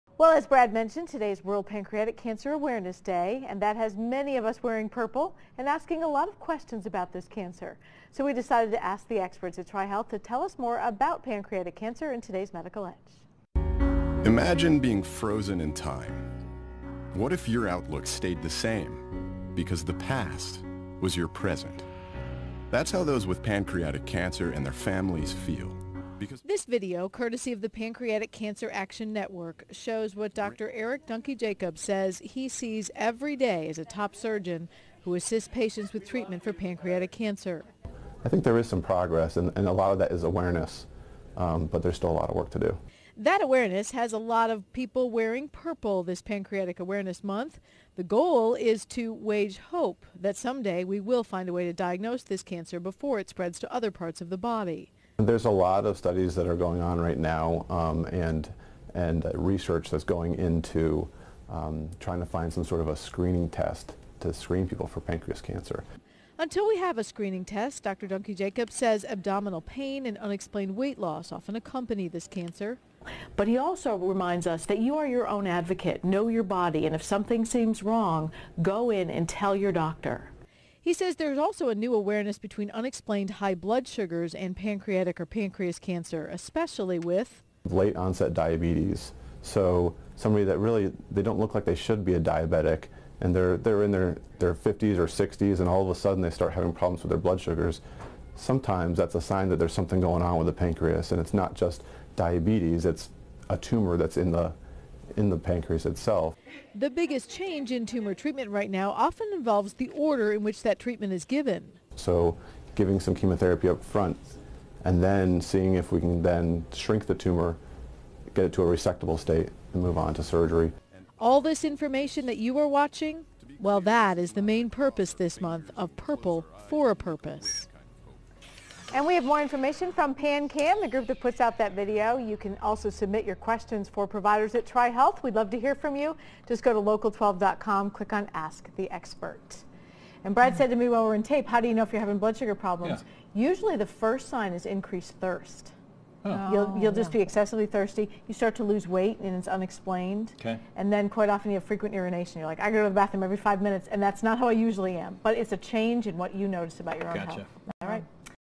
Ask the Expert: Why wear purple?
CINCINNATI (Liz Bonis) -- Thursday, Nov. 13, was world Pancreatic Cancer Awareness Day and that had many people wearing purple.

There were also asking questions about the cancer. Doctor Erik Dunki-Jacobs is a top surgeon who assists patients with treatment for pancreatic cancer.

"I think there is some progress. But a lot of that is awareness, and there's still a lot of work to do," he said.

That awareness had a lot of people wearing purple for Pancreatic Cancer Awareness Month. The goal was also to wage hope that someday doctors will find a way to diagnose the cancer before it spreads to other parts of the body.

Dr. Dunki-Jacobs said, "There's a lot of studies going on right now, and research that's going into trying to find some sort of a screening test to screen people for pancreas cancer."

Until people have a screening test Dr. Dunki-Jacobs said abdominal pain and unexplained weight loss accompany the cancer. He also said that people were their own advocate. Know their body and if something seems wrong go in and tell a doctor. He said there was a new awareness between unexplained high blood sugars and pancreatic or pancreas cancer.

Especially with, "Late onset diabetes. So someone that doesn't look like they should be a diabetic and they are in their 50s and 60s and they start having blood sugar problems